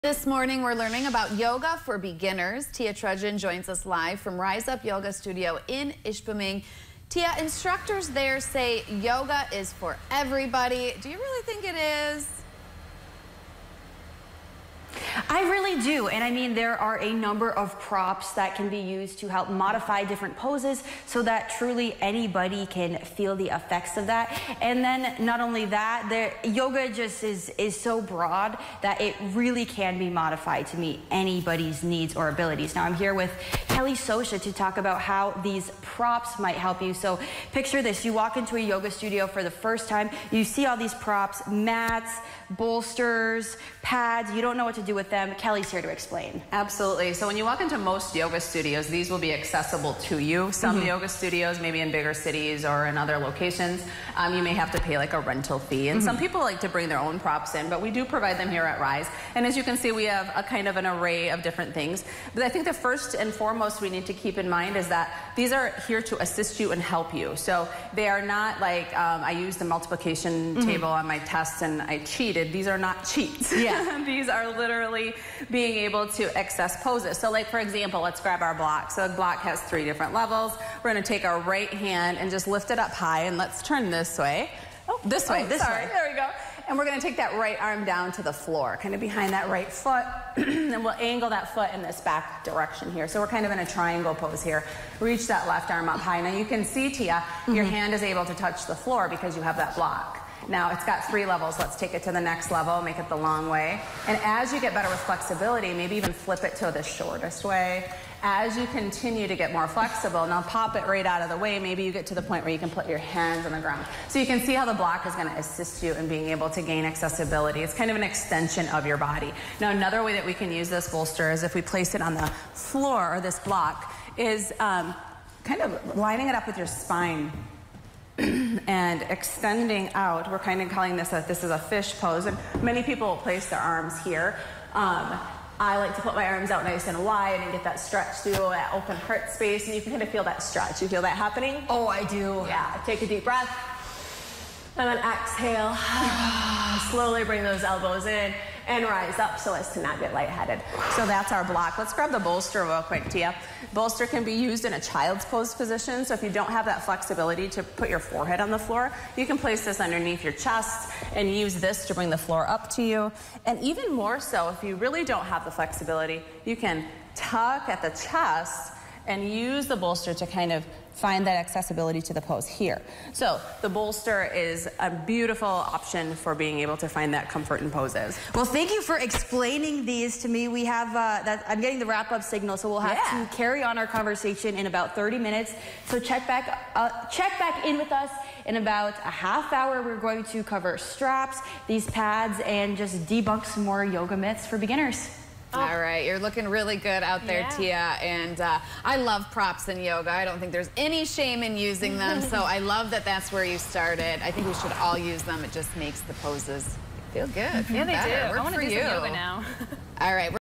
Wellness Wednesday - Yoga Basics
Rise Up Yoga's Kellie Socia debunks the yoga myth that using props means you are cheating.